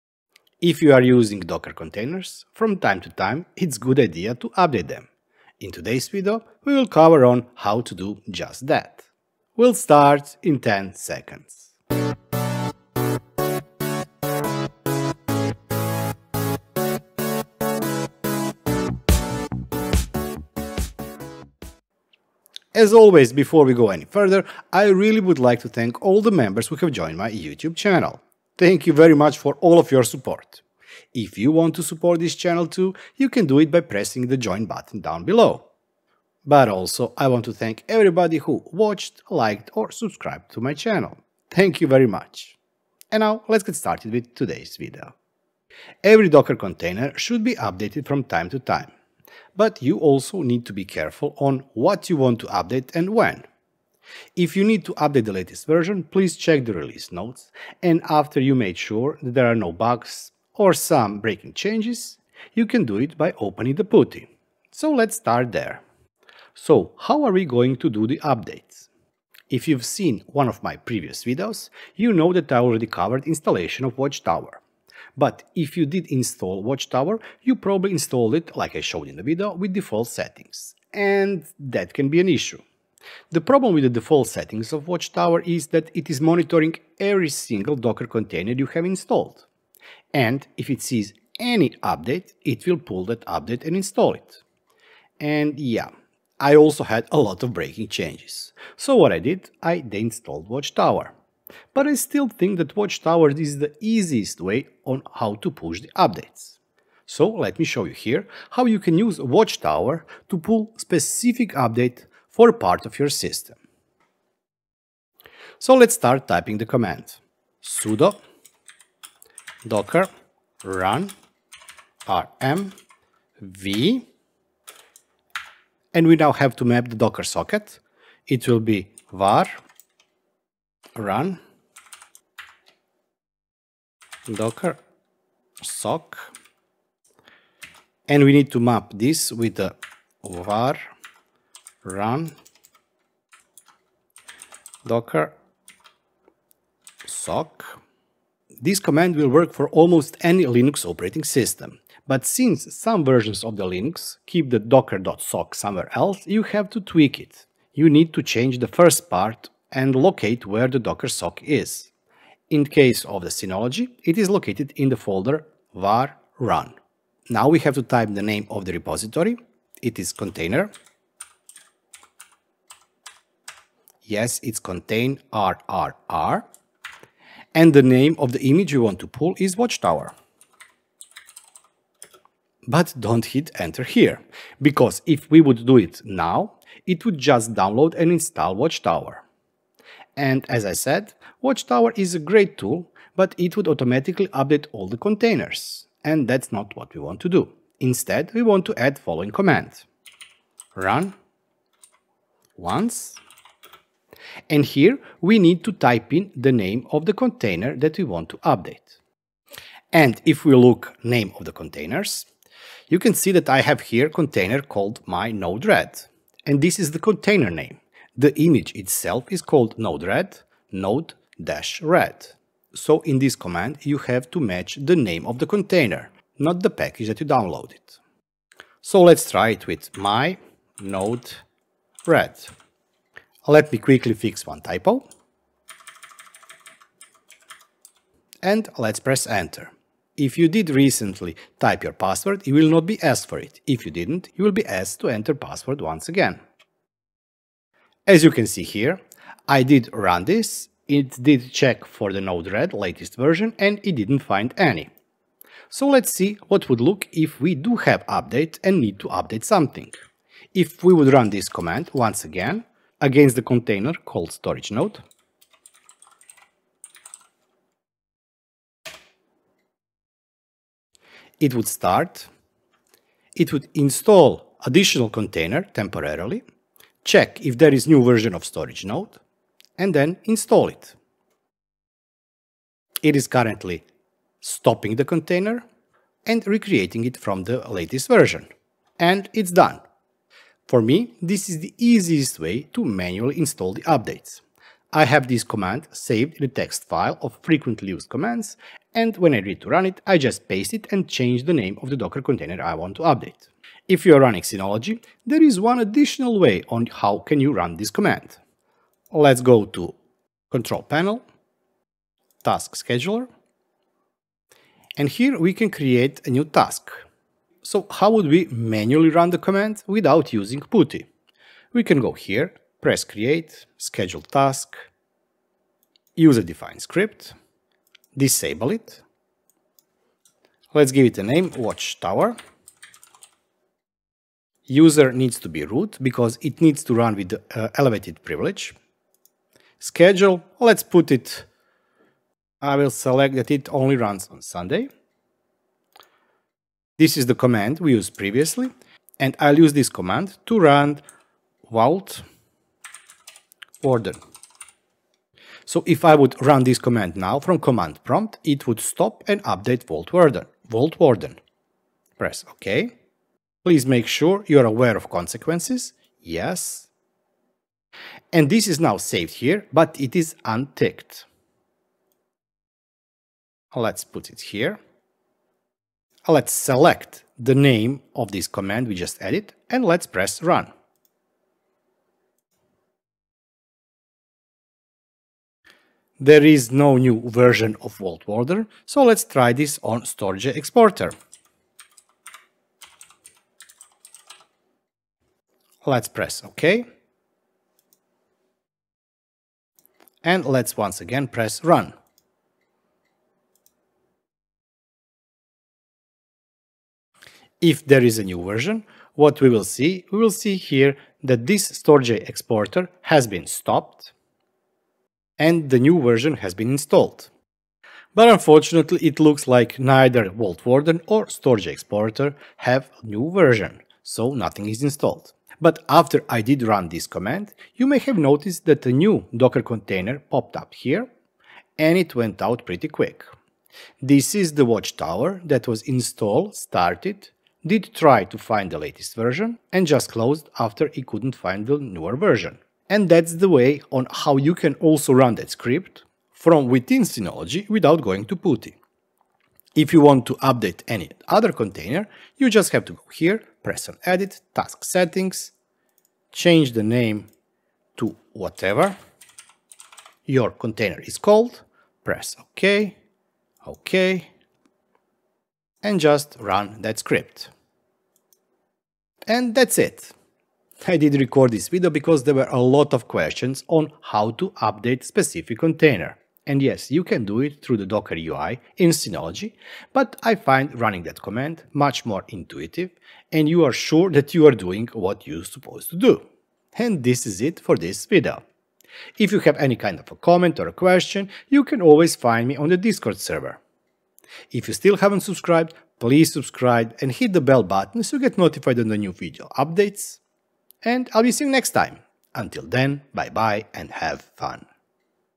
Manual update of Docker container on Linux & Synology with Watchtower
There are few different ways on how you can update your Docker containers. I find this one, that we'll cover today, easiest and that's the one I have been using for all my Docker container updates in last few years. And this is running Watchtower with --run-once command.

In the video I'll show you also how to run this command in Synology and completely skipping CLI.

Of course, you can always go for permanent Watchtower installation, but then you also risk a chance of getting unwanted upgrades - that sometimes can break you system.

⏵ Update command:
sudo docker run --rm -v /var/run/docker.sock:/var/run/docker.sock containrrr/watchtower --run-once storagenode

00:00 Intro
00:21 Thank you to channel members
00:45 Updating container
01:46 Let's use Watchtower again
05:03 Alternative way for Synology users
07:50 Finals thoughts

Have fun!
BeardedTinker

🧡 If you would like to support me and this channel, you can JOIN and become channel member on YouTube! 🙏
📣Follow me on other platforms📣